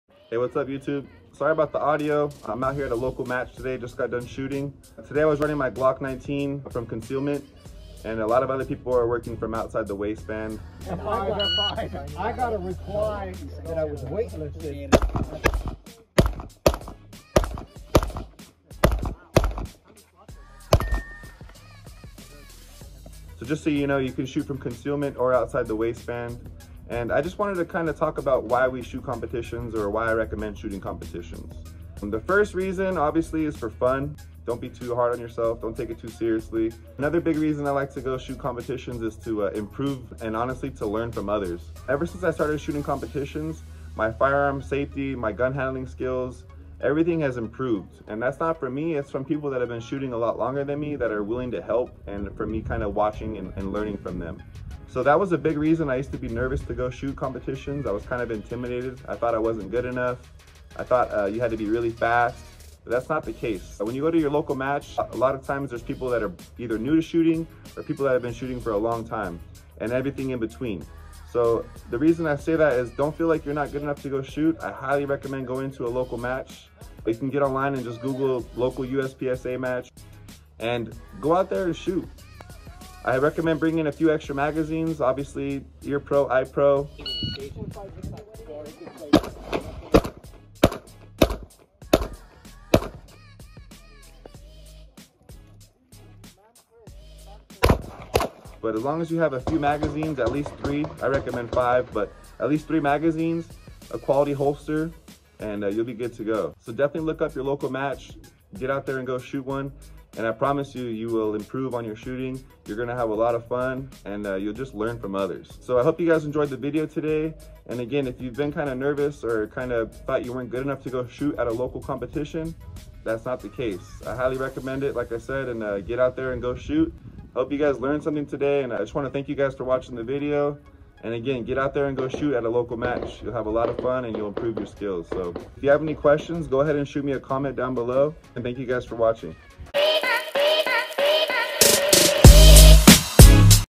Easy Way To Improve Your Shooting Skills
I go over the reasons why I like to shoot competitions and explain why I encourage you to go shoot in a match. They are a lot of fun and I promise it will improve your skills and safety all around.